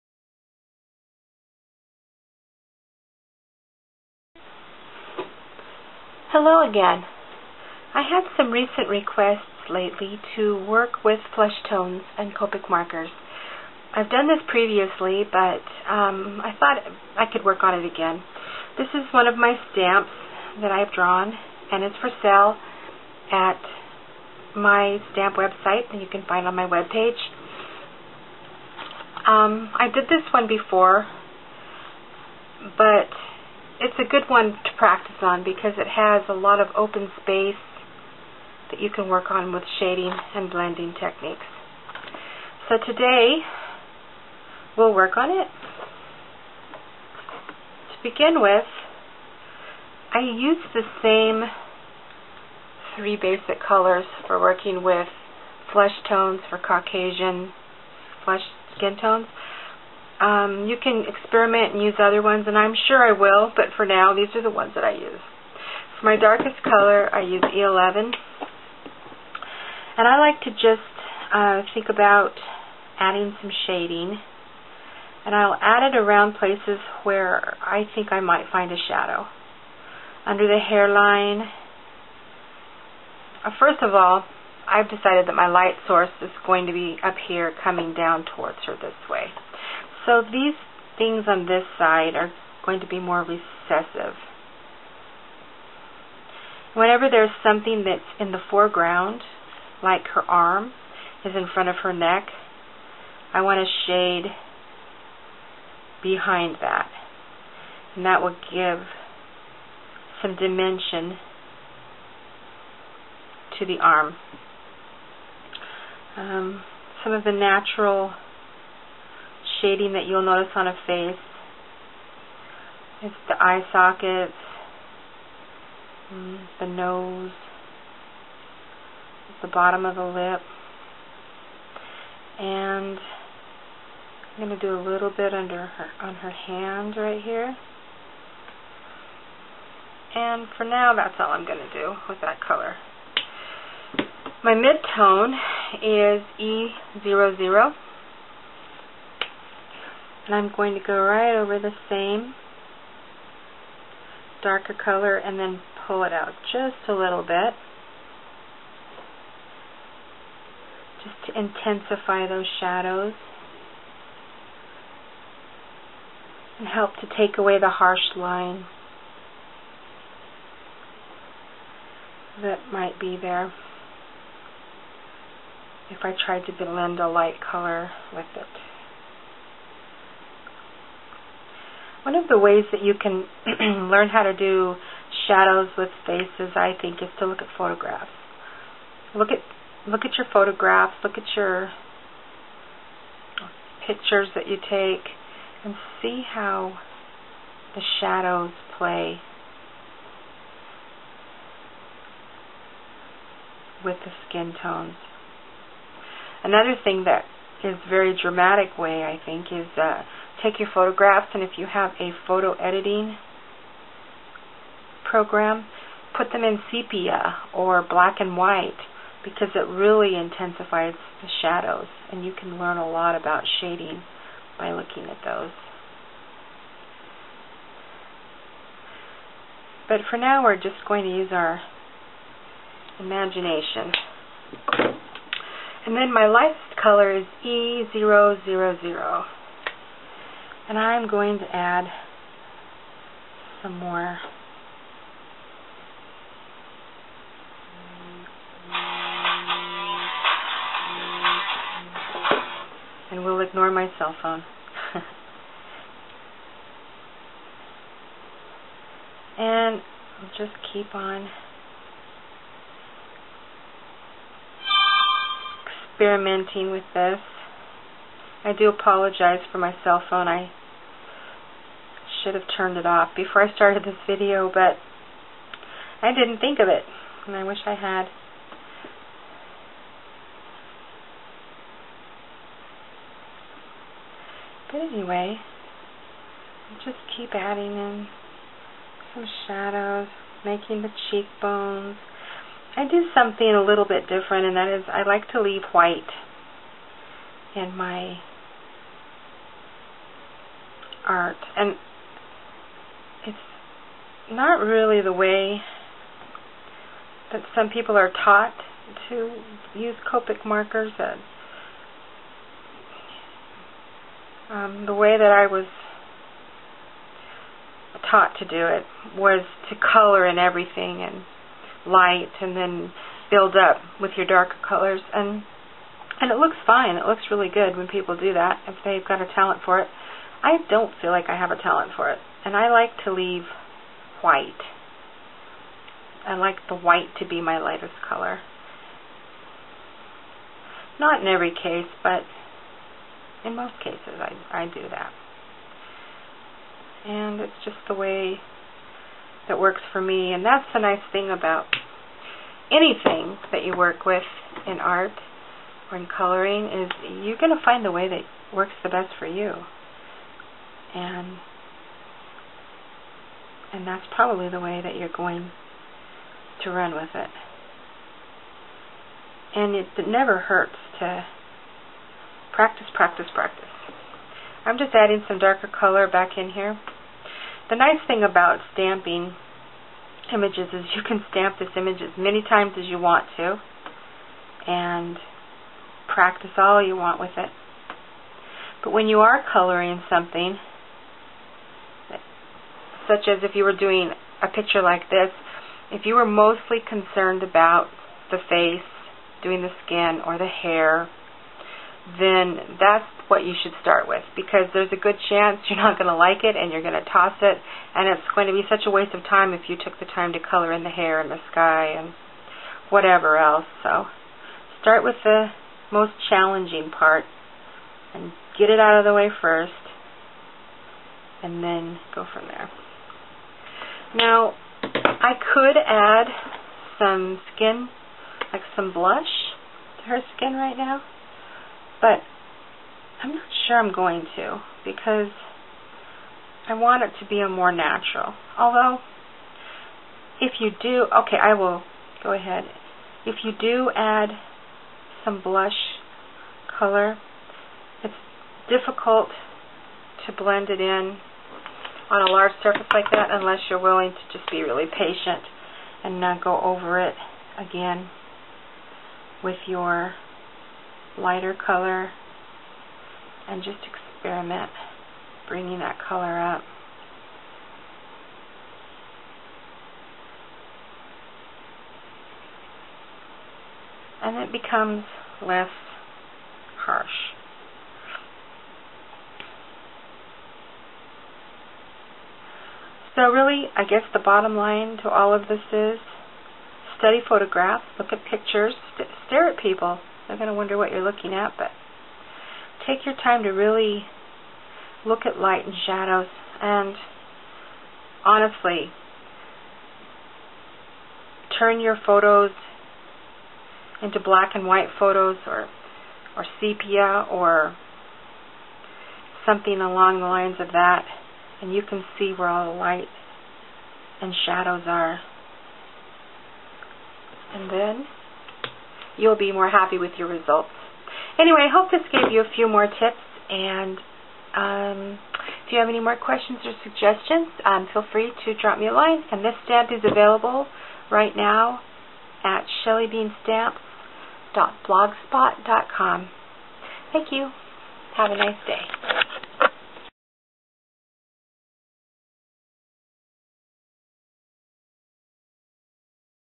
Coloring Flesh tones #2
Coloring flesh tones using Copic Markers.  The stamped image used in this video is available for sale